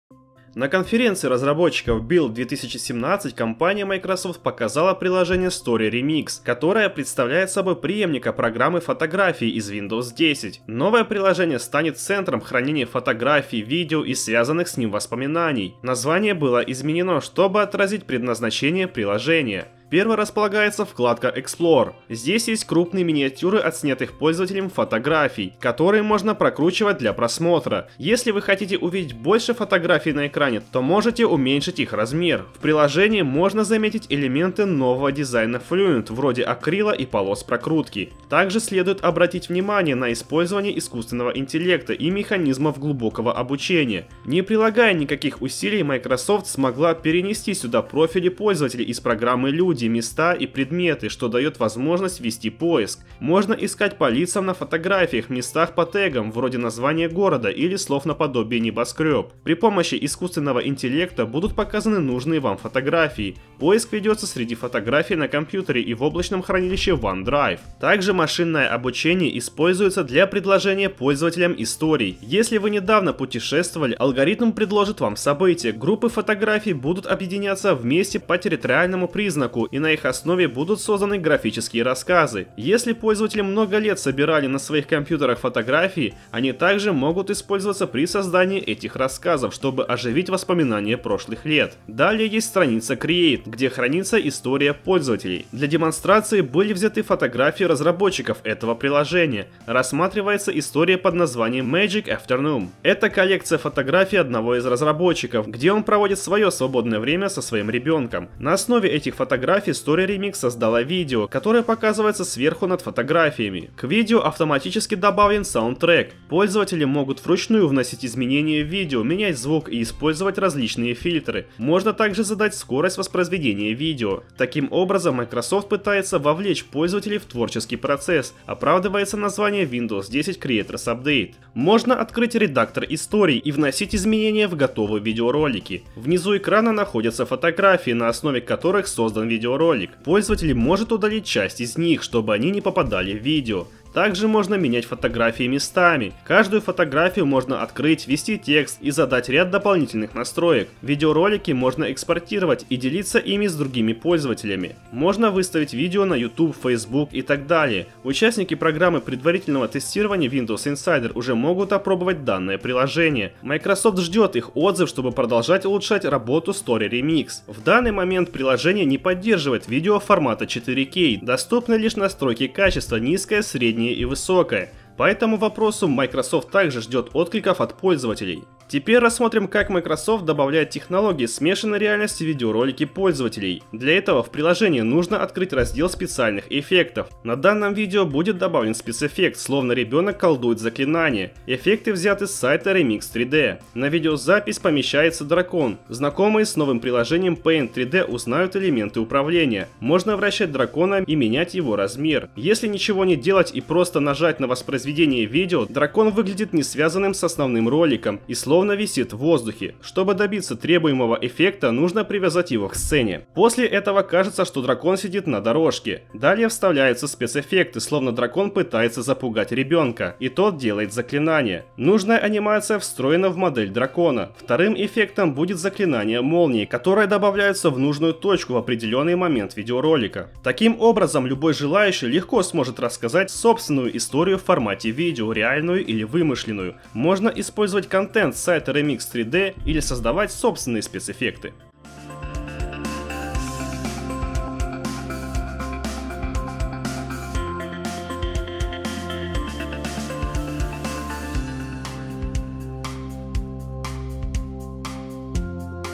Story Remix – Видеомонтаж силами самой Windows 10
Windows Story Remix – встроенная программа для видеомонтажа.